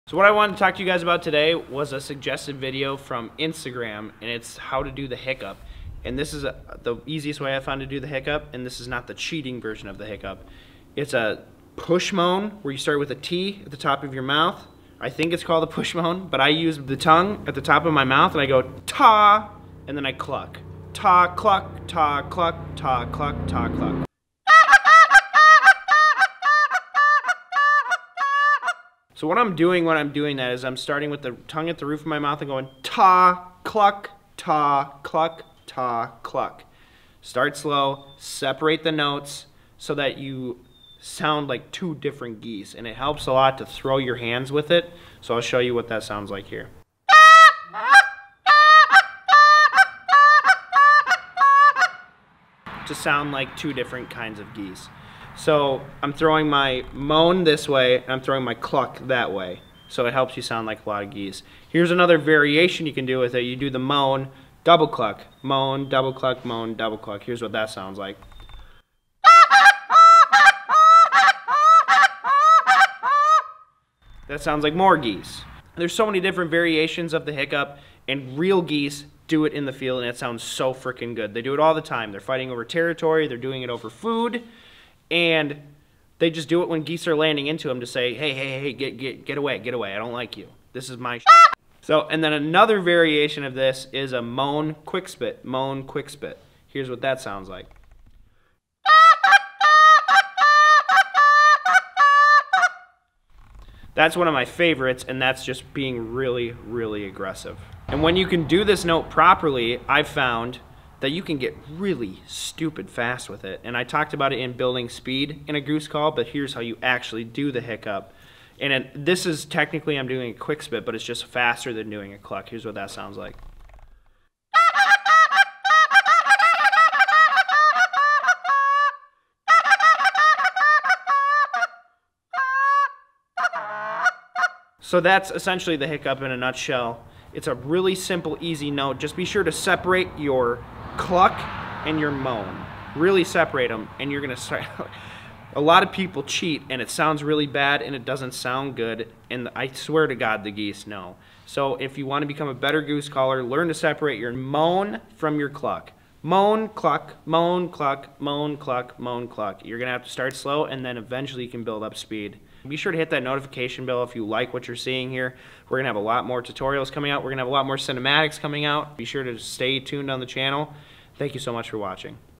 How to do the Hiccup on a Goose Call
Thank for all your questions. Here is another viewer submitted request. How to do the goose hiccup. Enjoy!

Use